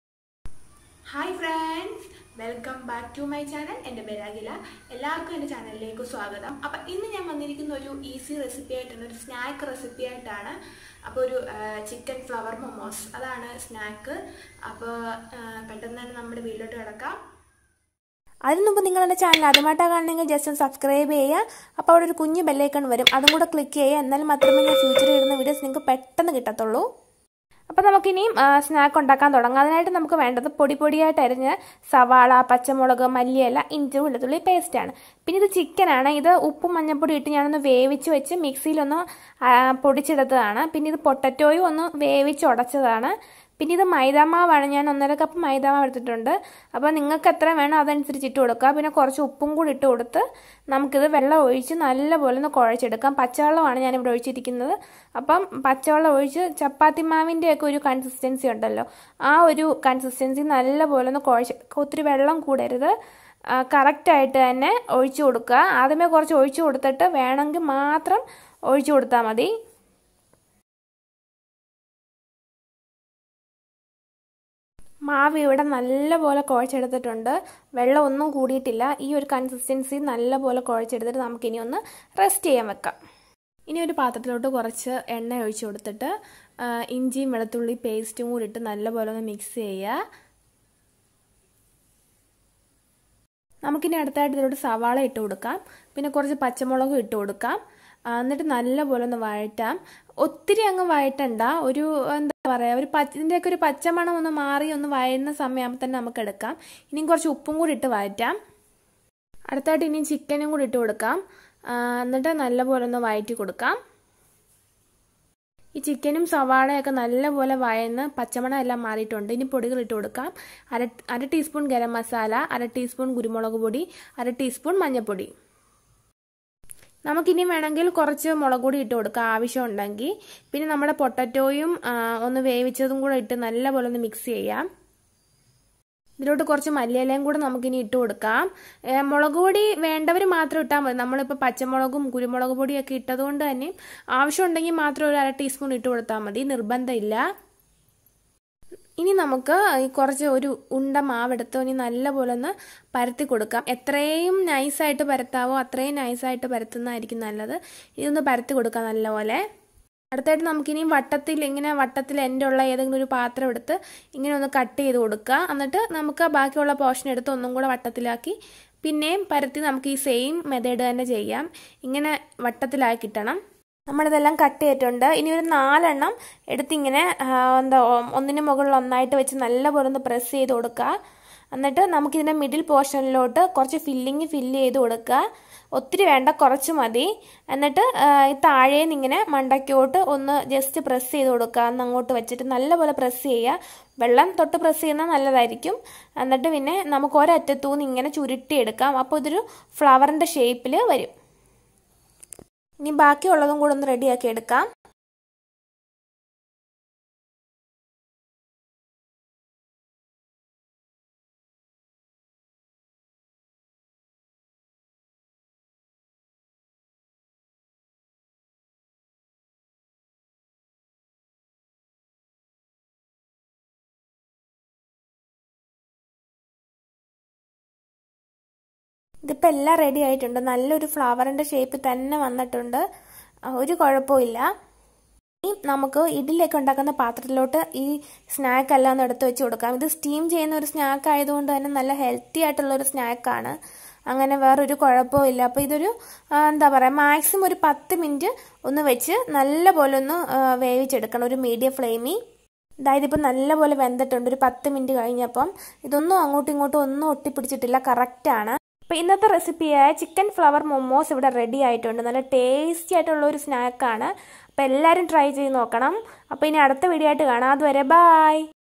Chicken flower momos|Evening snack |Healthy snack
This vedio show that how to prepare chicken flower momos. It is a healthy steamed snack.

Ingredients

Maida
Oninon
Chillly
Ginger garlic paste
Corriander leaves
Chicken
Potatao
Grama masala
Pepper powder
Turmeric powder
Chilly powder
Salt